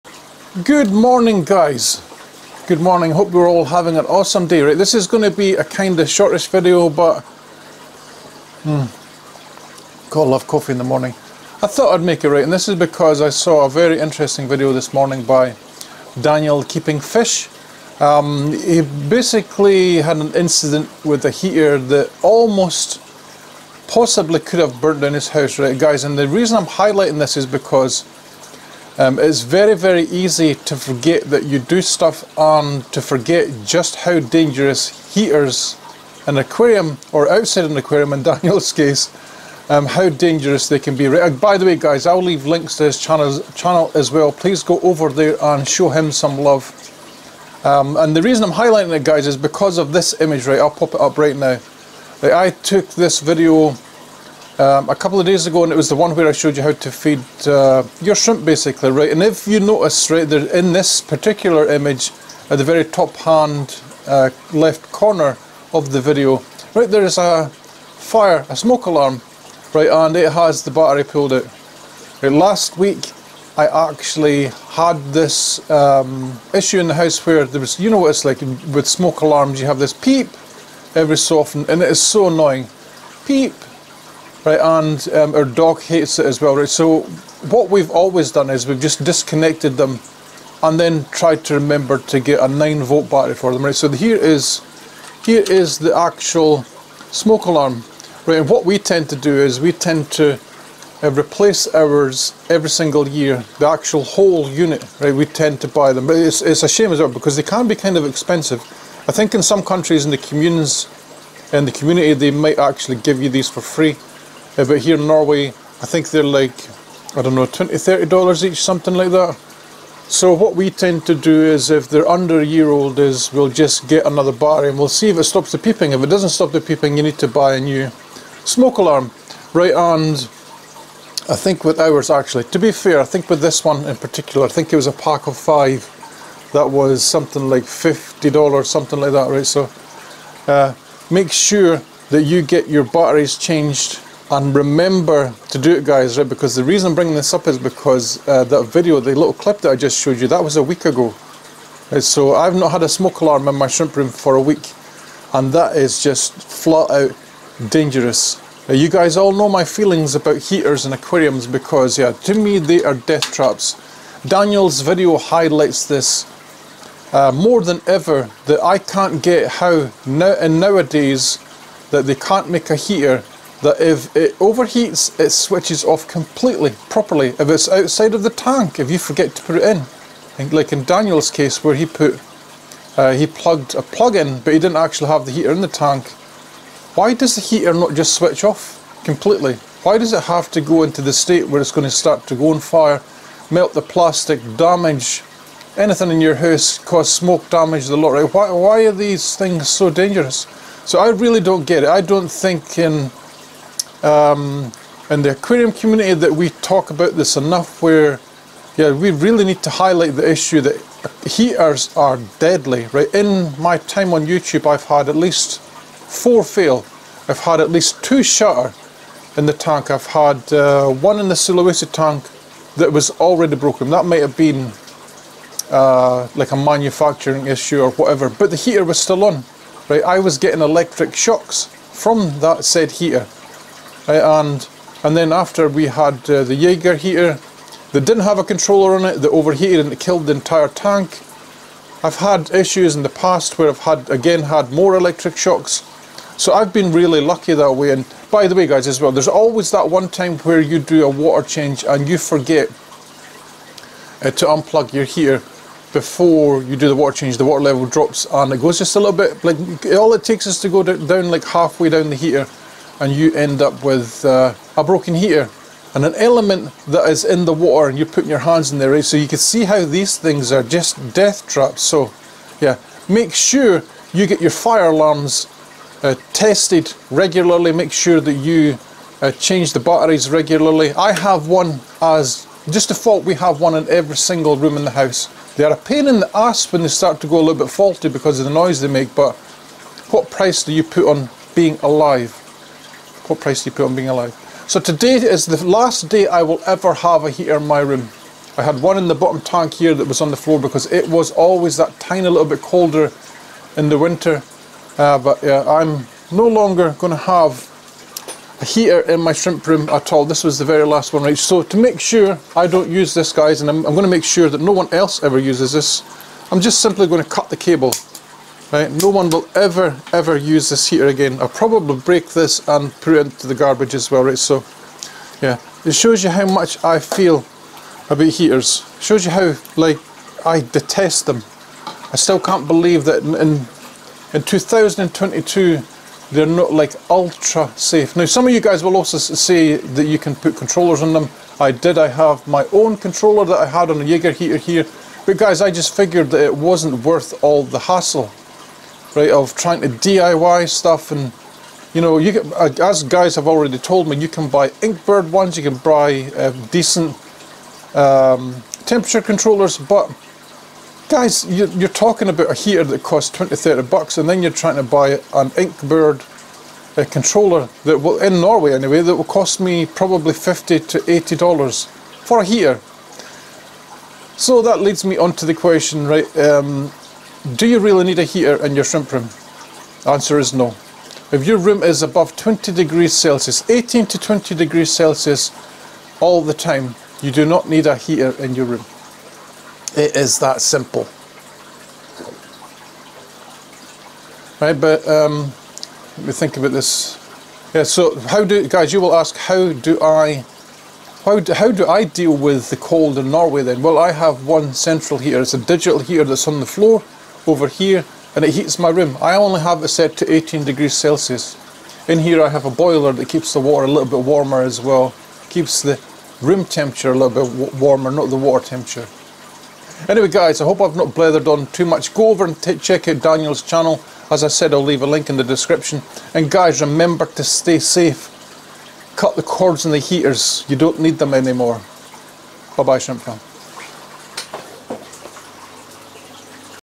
Your aquarium heater could burn your house down
How to fix aquarium heater fire hazards and safety risks by choosing room heating over dangerous submersibles.

Want step-by-step shrimp guides and less guesswork? ✅
📘Neocaridina for Dummies (instant download) 📥

📙The Ultimate Guide To Breeding Caridina Shrimp (instant download) 📥